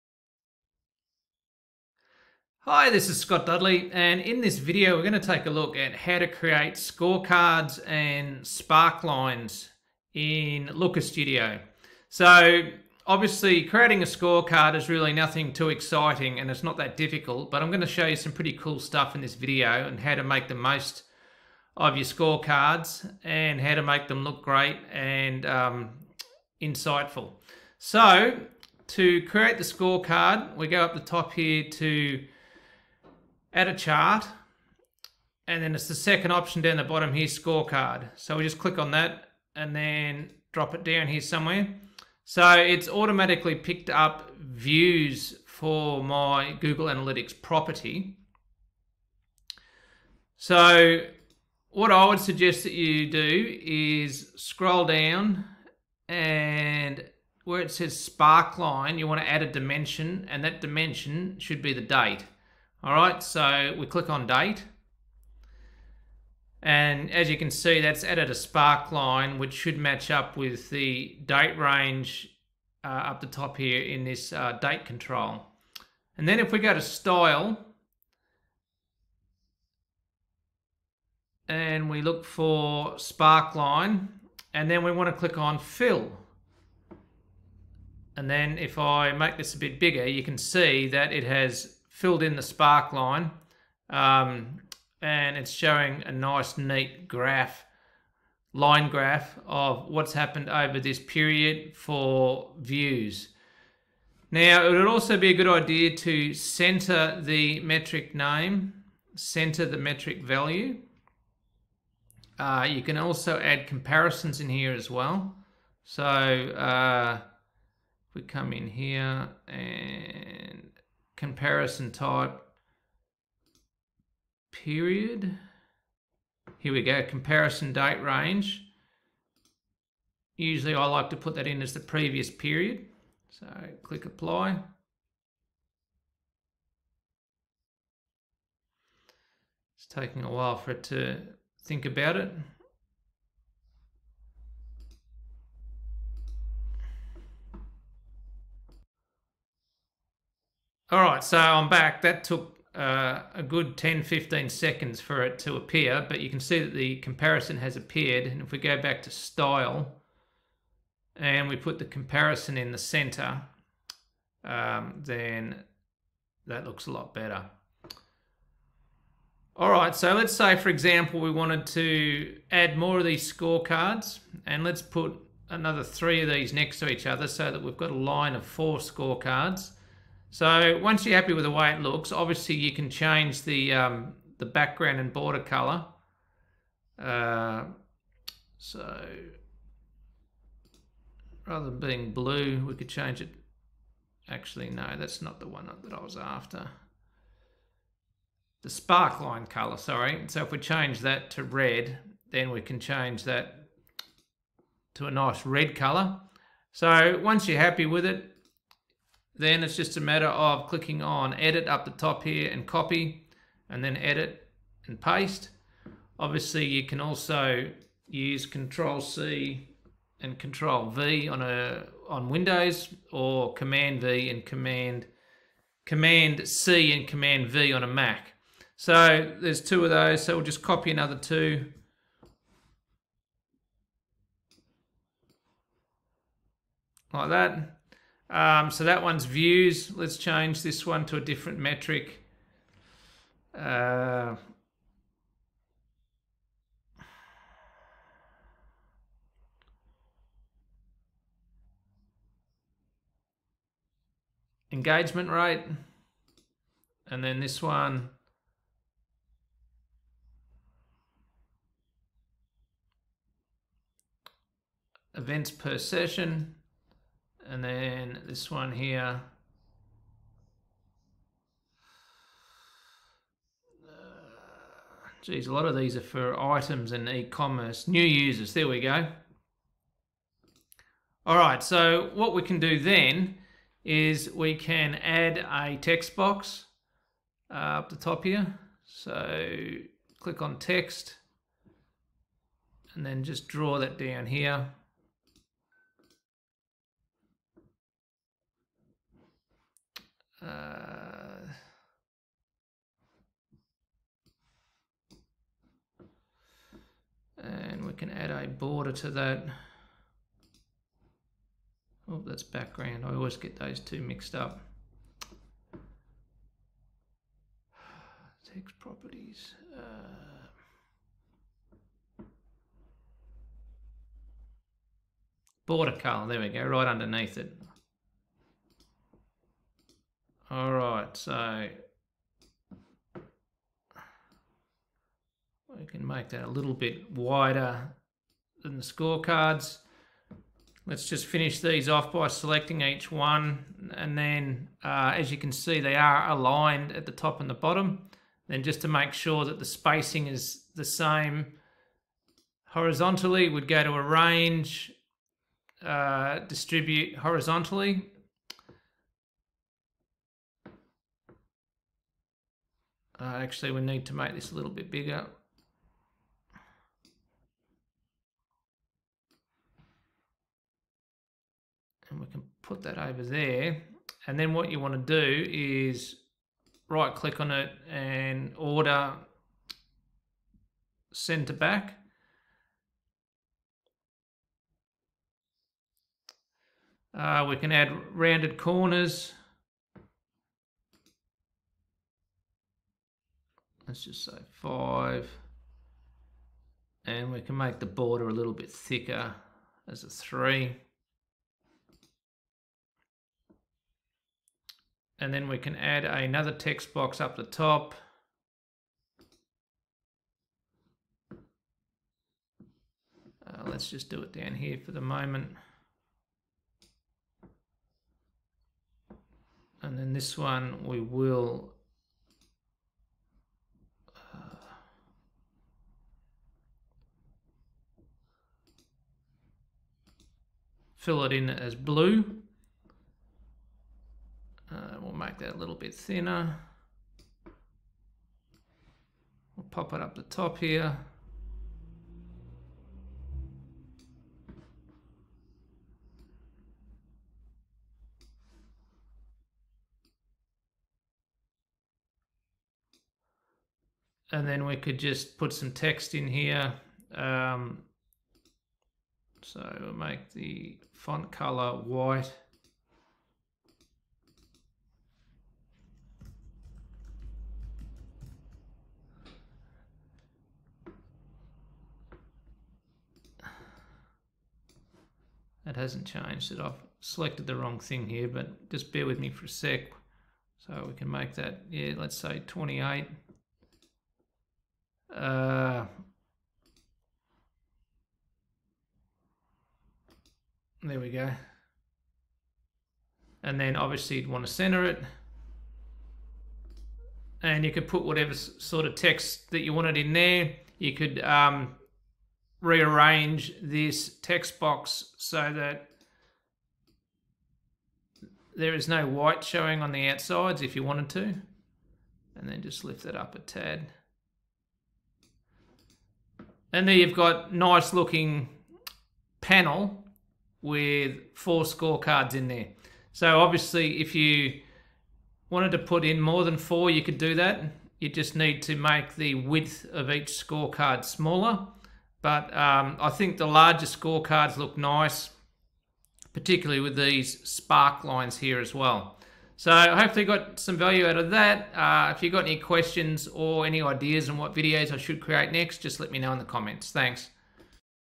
How to Create Awesome Scorecards with Sparkline Graphs in Looker Studio
Get your FREE strategy session call to discuss customised Dashboards And Data Pipelines

Discover how to effectively create scorecards with dynamic sparkline graphs in Looker Studio. Scorecards provide a quick overview of key metrics, while sparkline graphs add a valuable historical context to your data. In this tutorial, I'll guide you through the process of creating engaging scorecards with embedded sparkline graphs, enhancing your data visualisation capabilities.

The blog post version of this video can be found